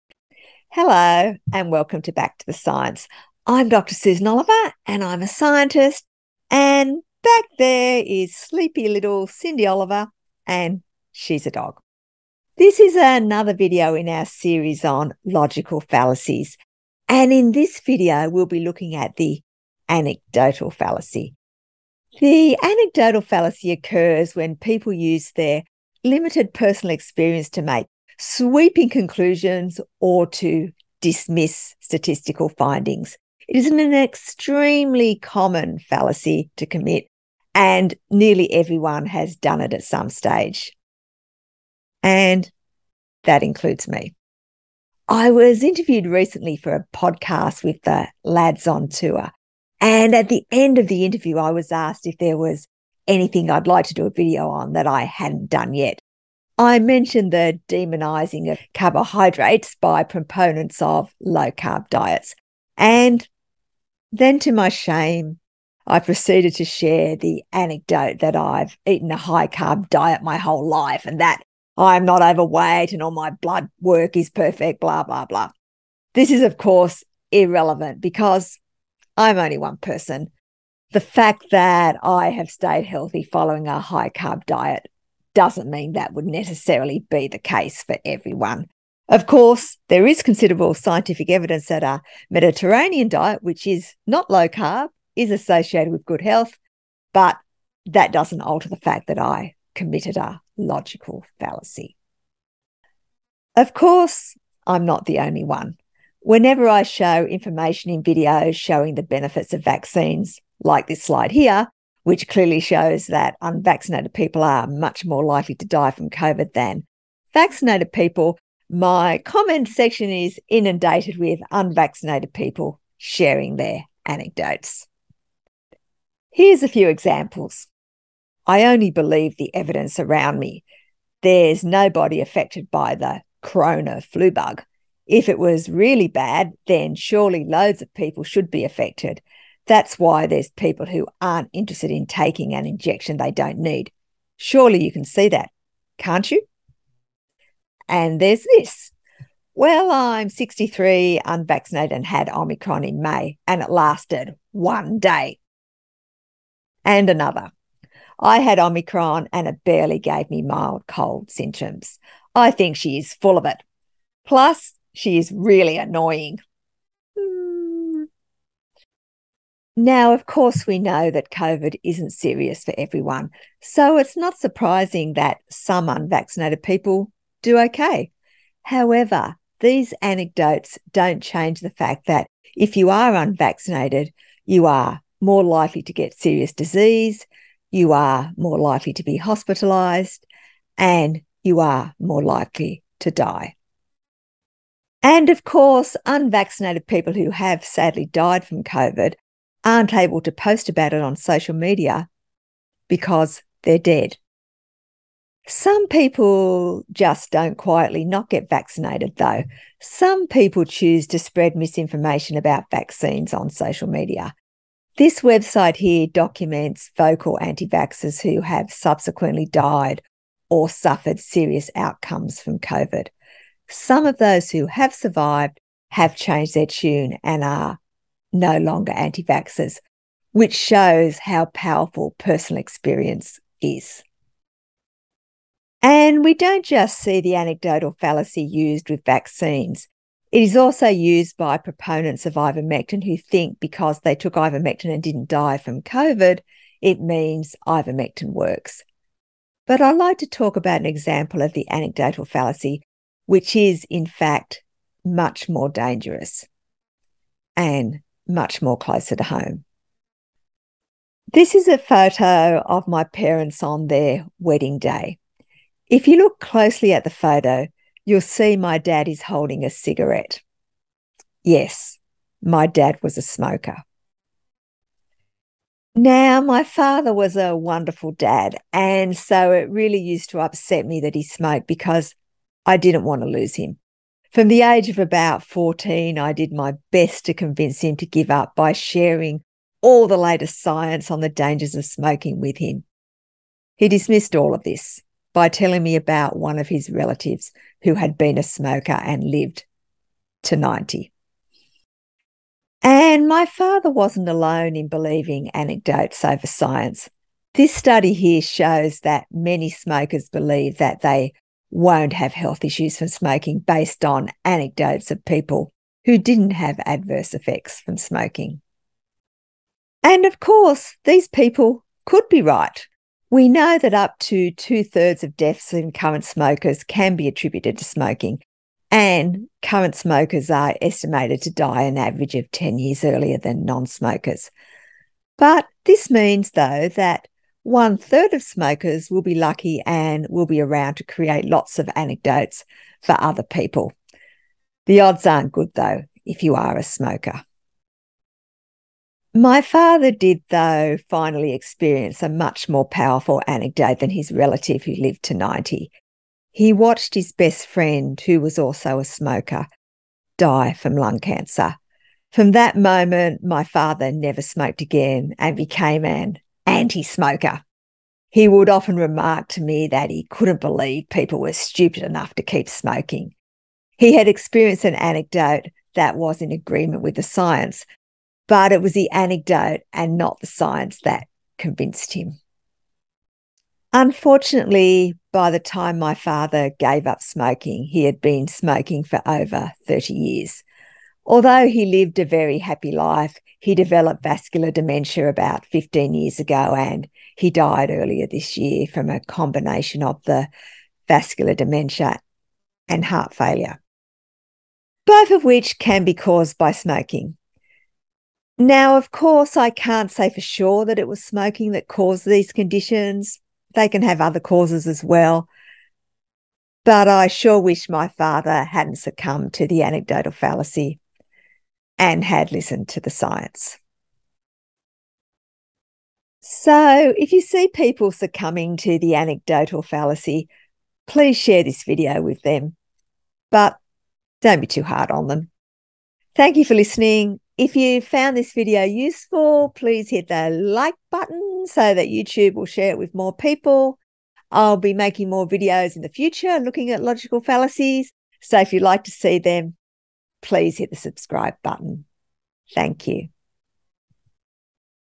Logical fallacies EXPOSED! #2 The anecdotal fallacy
Many people use logical fallacies in arguments. This is particularly common on social media. In this video, Dr Susan Oliver and Cindy the dog will be discussing the anecdotal fallacy. The anecdotal fallacy occurs when people use their limited personal experience to make sweeping conclusions or to dismiss statistical findings.

Links to data:

What are the effects of smoking and tobacco?

Smokers’ accounts on the health risks of smoking: Why is smoking not dangerous for me?